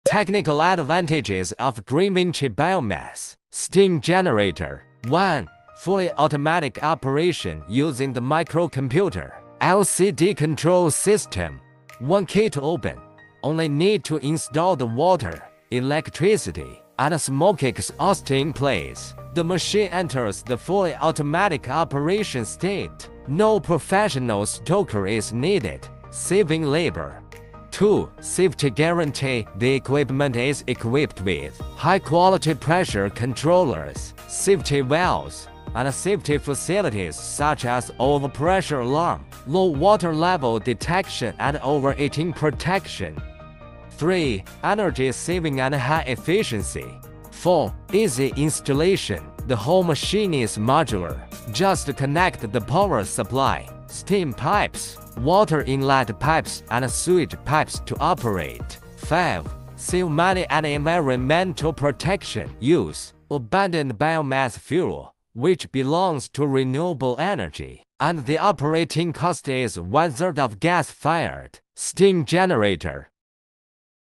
Steam generator technical advantages China manufacturer
technical advantages:
1.Wide range of steam output :simple machine at least 5KG/H,maximum 1000KG/H
2.Wide range of applicable fuels,choose approiate fuels according to local energy advantages